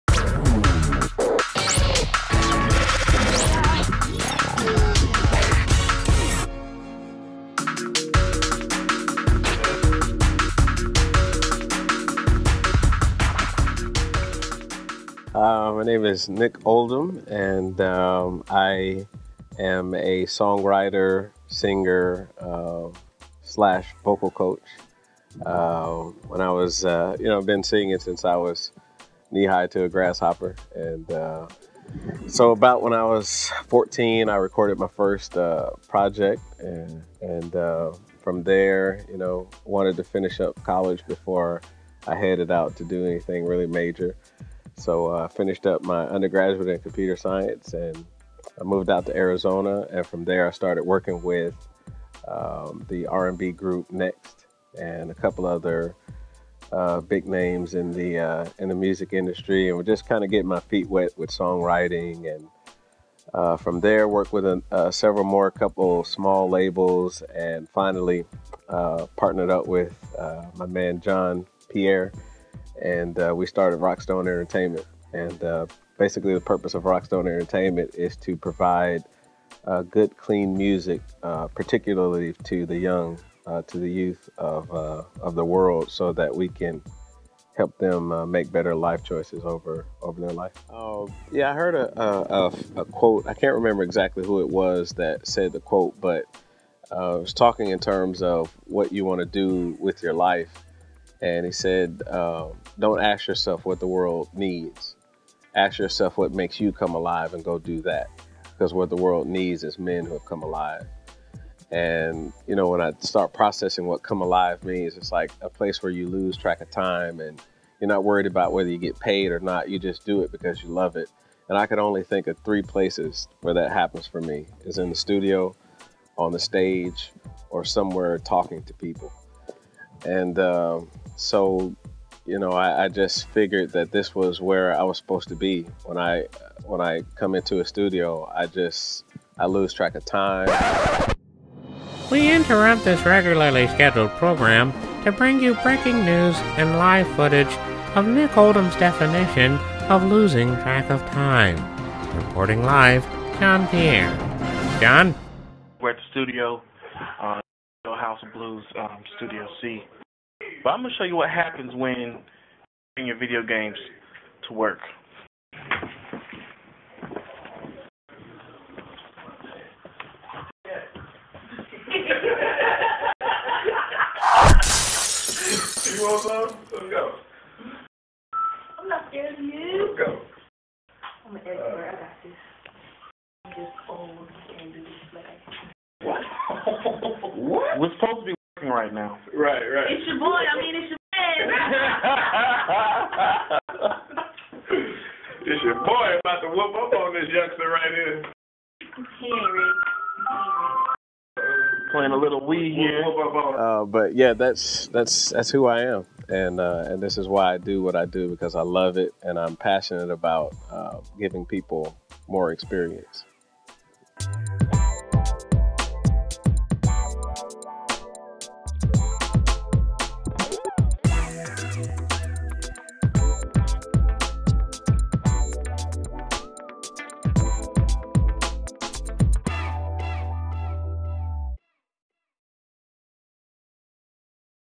RockStone Friday Feature
Nick Oldham - singer, songwriter & vocal producer - explains how he got started with RockStone Entertainment and why studio life is the life for him. He really looses track of time while at the studio (wink, wink).